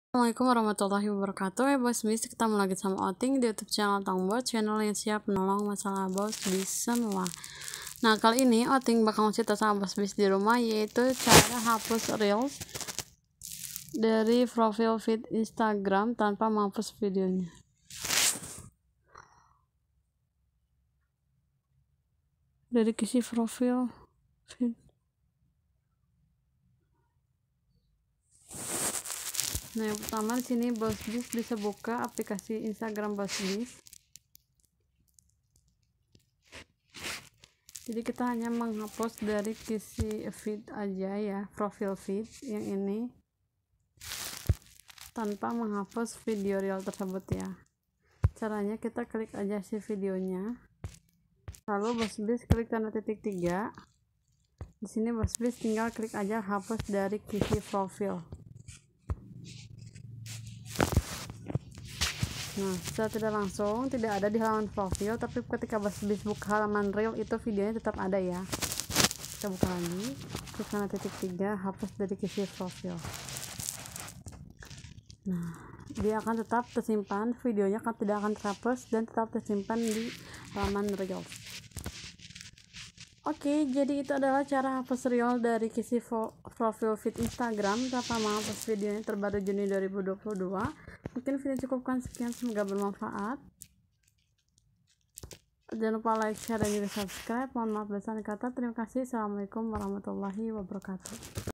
Cara Hapus Reels Dari Kisi Profil Feed Instagram Tanpa Menghapus Videonya Terbaru Juni 2022
Hallo bos bis dalam video ini otong bakal sharing ke Bos Bis semua, Cara Hapus Reels Dari Kisi Profil Feed Instagram Tanpa Menghapus Videonya Terbaru Juni 2022,    bagi bos bis yang belum tau yuk simak videonya dan jangan di skip ya biar paham, mudah mudahan bermanfaat :)